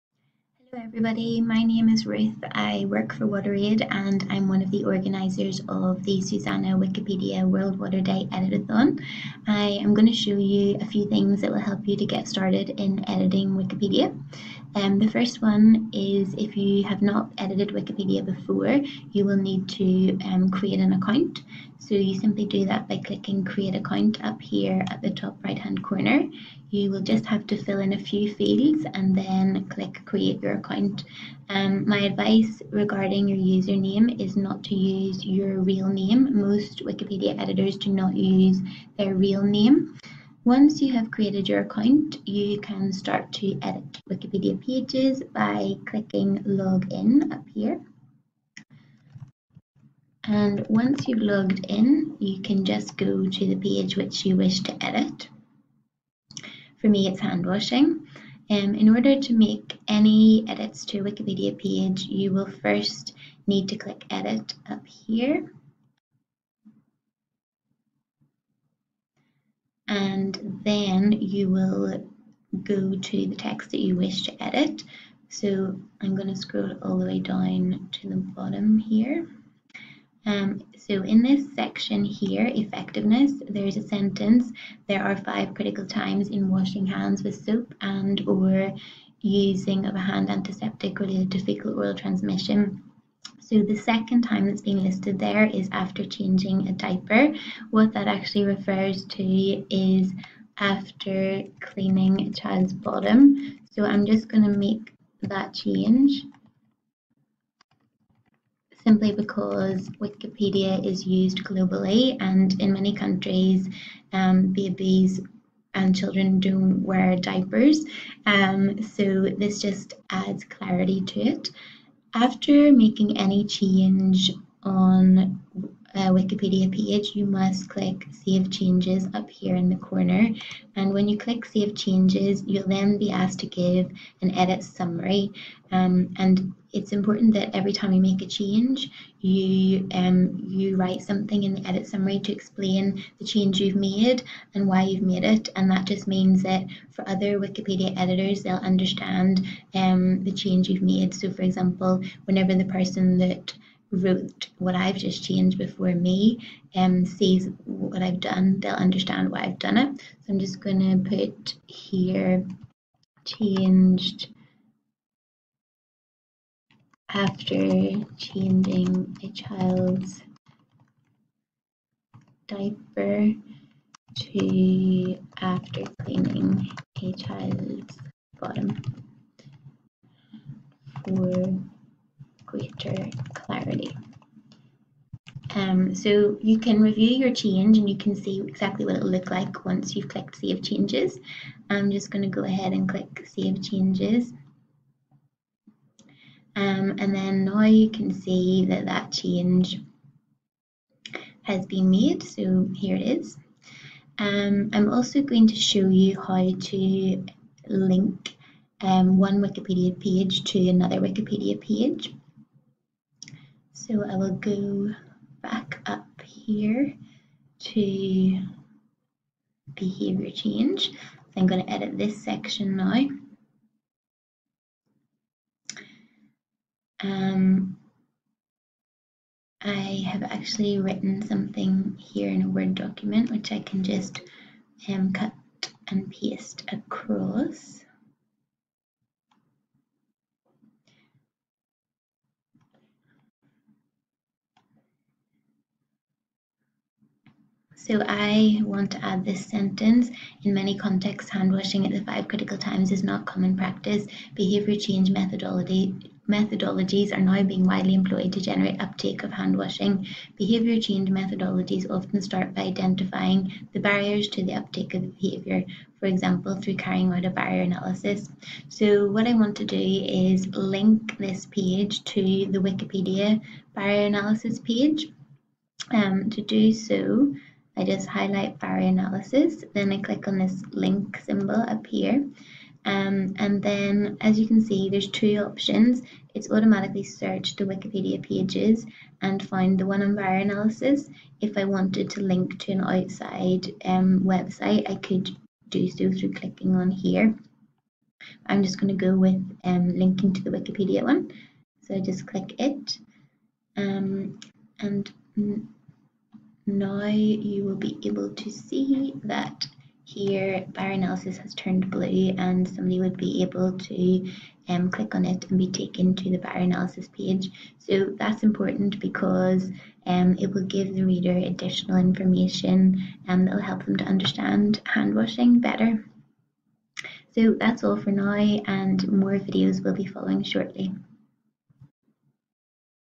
How to make your first edits in Wikipedia
Video on how to edit-a-thon for the World Water Day (WWD)

Video created to support Sustainable Sanitation Alliance (SuSanA) Edit-a-thon for World Water Day March 2017 @ SuSanA Platform Worldwide

Video by Ruth Miskelly, WaterAid